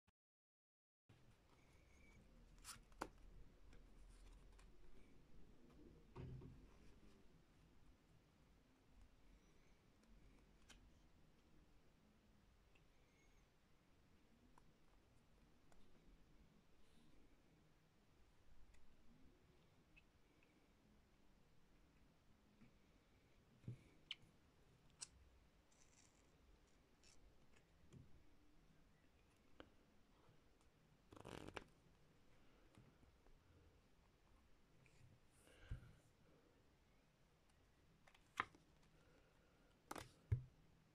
Palm transfer (right classic to left cop) | slow mo tutorial
This is just an idea I had a few weeks ago.

Just to be clear : THIS IS NOT POLISHED YET
This is (as said two sentences back) just an idea.

If you want to add anything, be my guest. (the only thing that I'm asking though, is to not take all of the credit 😋)

This is the same concept as the squeeze shift. That's why I would like to only take the credit for the idea of a palm to palm transfer (because, no, I've never seen anything like it).